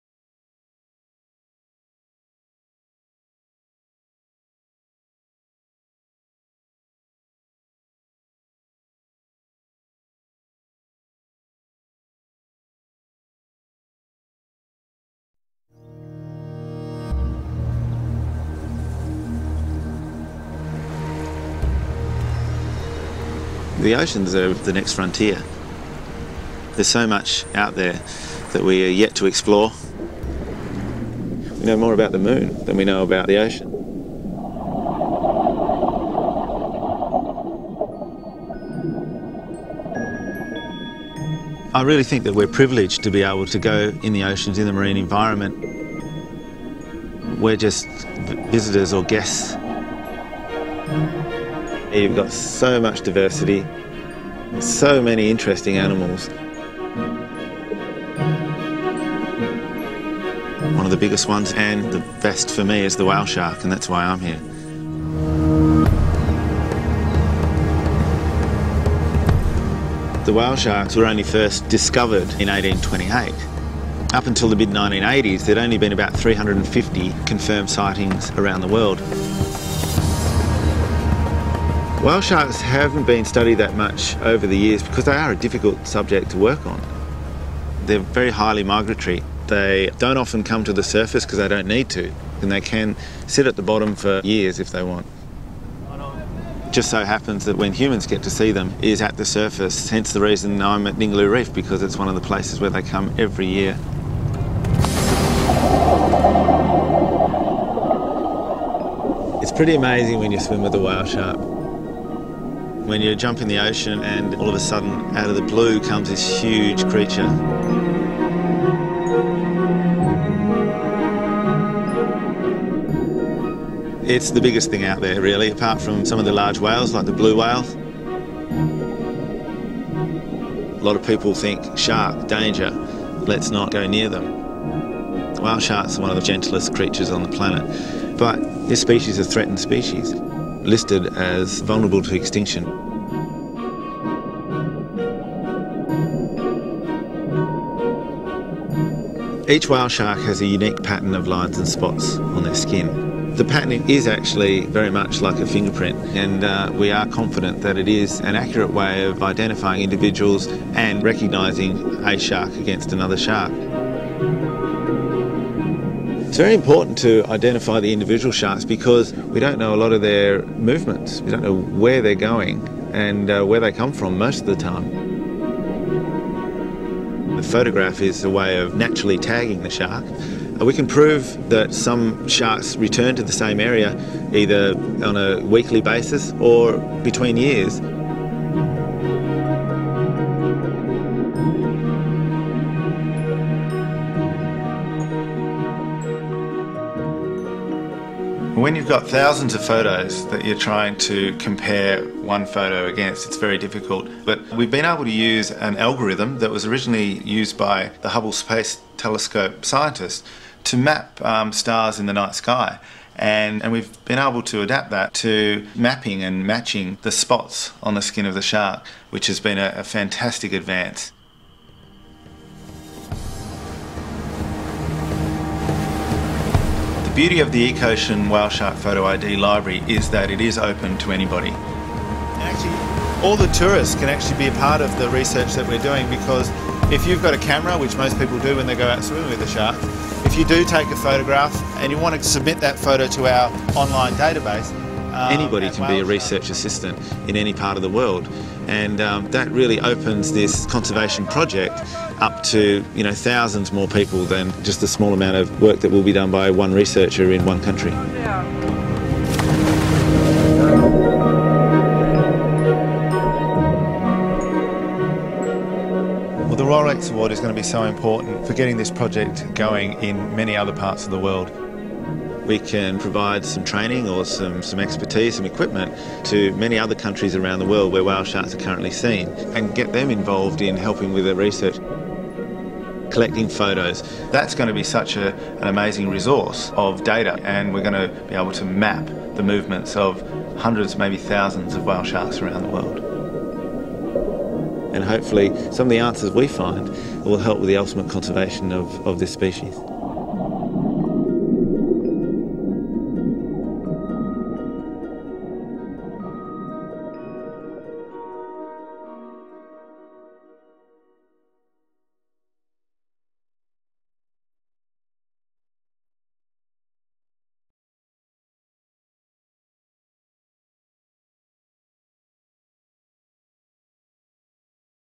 Brad Norman, Rolex Laureate, 2006
Impelled by a love of the sea and its largest fish, the elusive whale shark, marine conservationist Brad Norman has created a photo-identification system to assist its conservation.